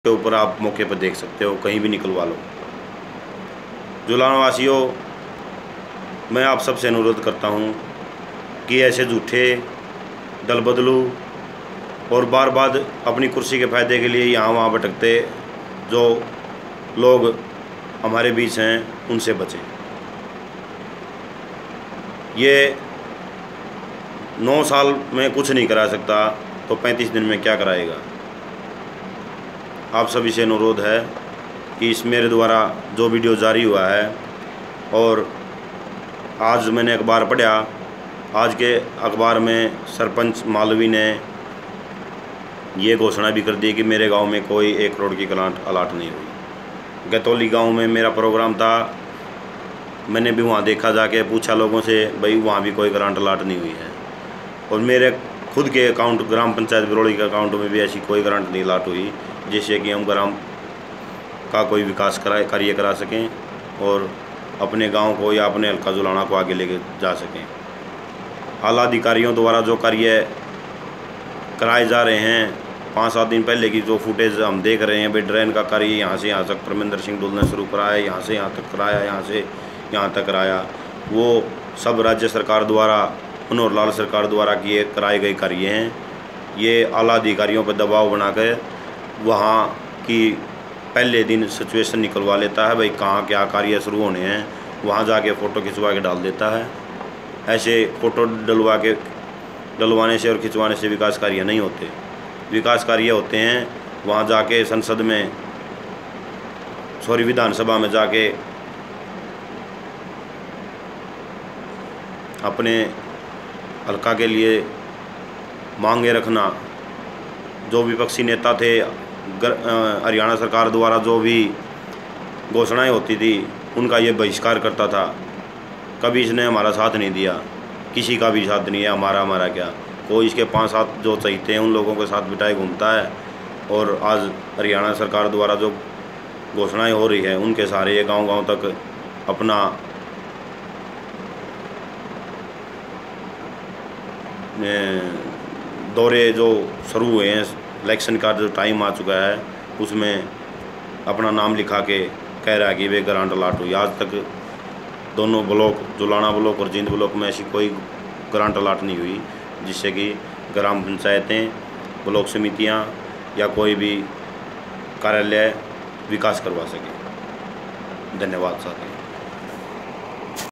भाइयो परमेन्द्र दल पदलू हैं । सिर्फ अपने फायदे के लिए पार्टी बदल ली । सरपंच बिरौली
यो सिर्फ अपना भाविष्य देखे ह। जूठे ग्रांट क्वे ह दिए ह पर सरपंच बिरौली सबूत ह कोई ग्रांट नही आया ह। भाइयो सोच समझ लो इब तो।